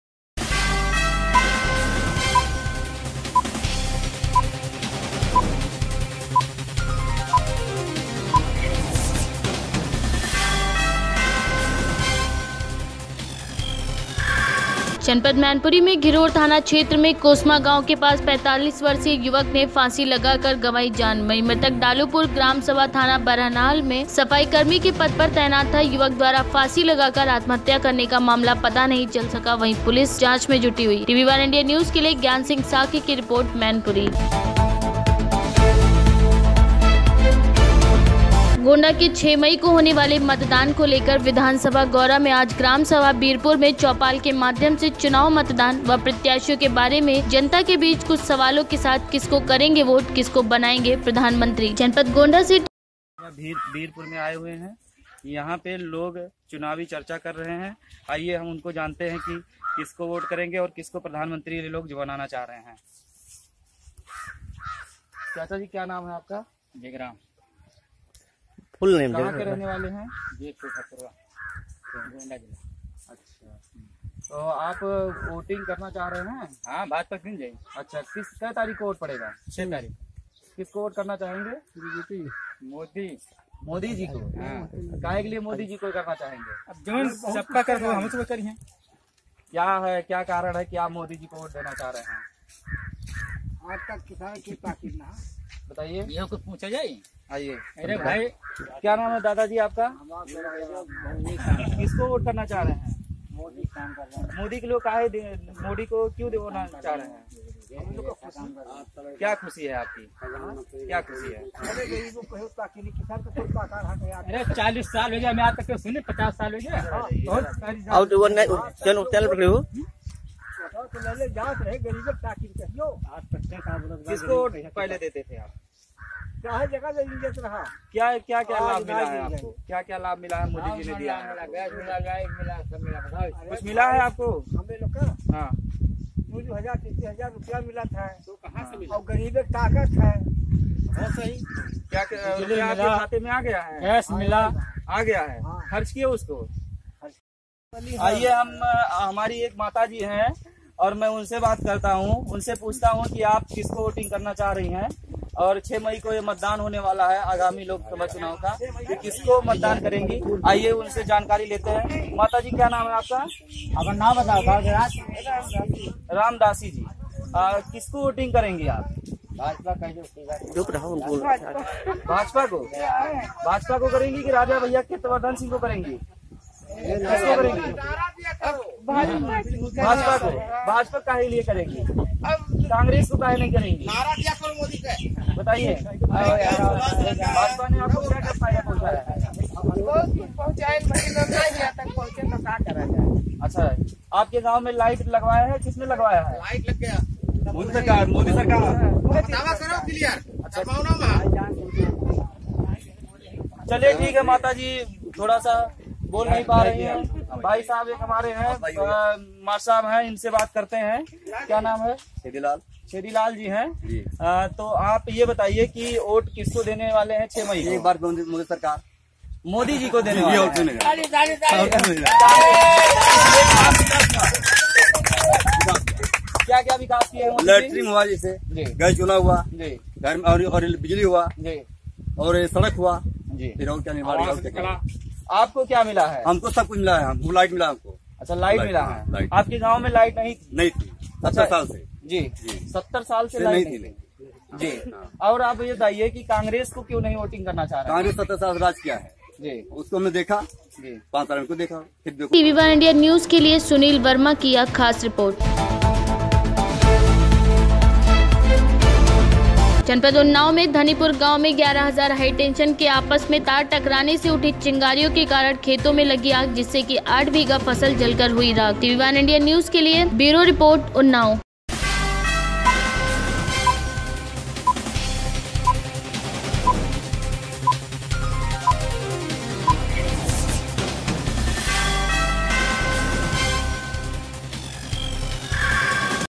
tv1indianews-गोंडा के विधानसभा गौरा में चौपाल के माध्यम से जनता से किये कुछ चुनावी सवाल
ज्ञान सिंह शाक्य -मैनपुरी
सुनील वर्मा -गोंडा
ब्यूरो रिपोर्ट -उन्नाव